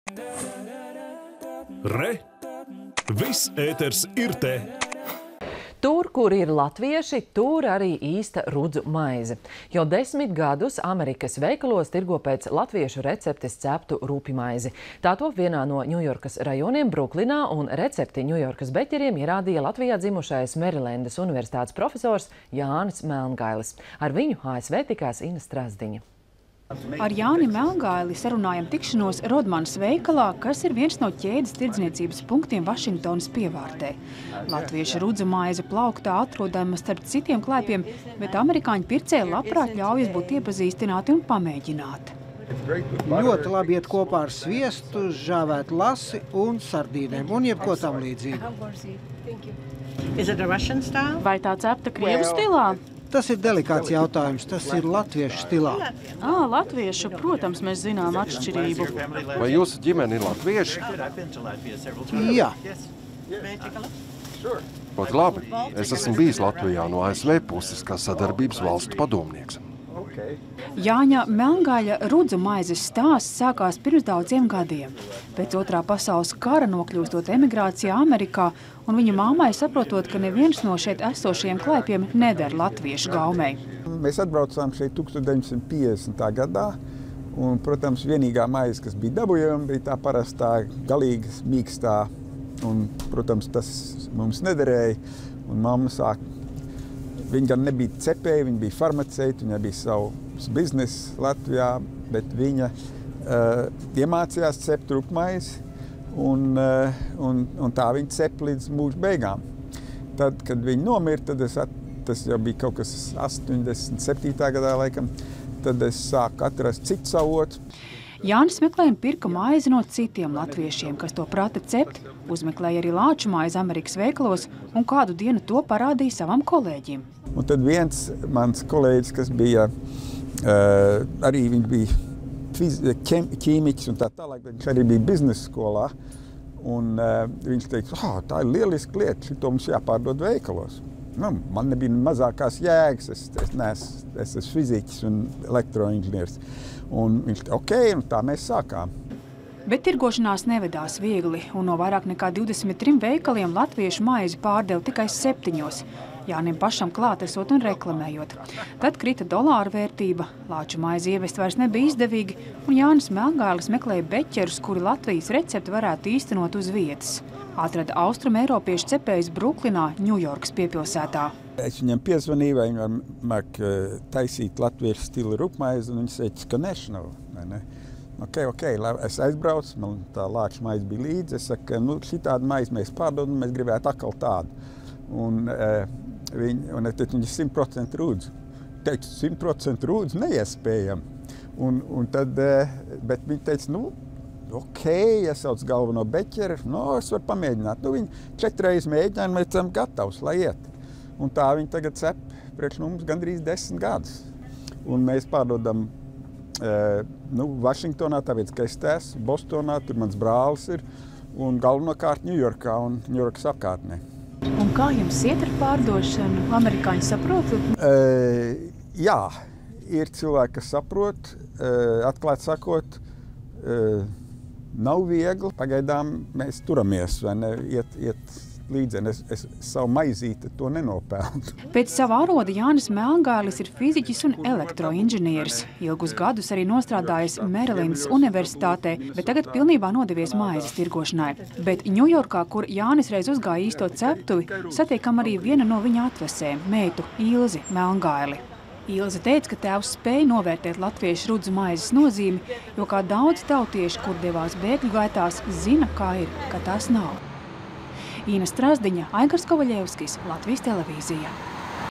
Latviešu rudzu maize ASV
Tur, kur ir latvieši, tur – arī īsta rudzu maize. Jau desmit gadus Amerikas veikalos tirgo pēc latviešu receptes ceptu rupjmaizi. Tā top vienā no Ņujorkas rajoniem – Bruklinā. Recepti Ņujorkas beķeriem ierādīja Latvijā dzimušais Merilendas Universitātes profesors Jānis Melngailis. Ar viņu ASV tikās Ina Strazdiņa.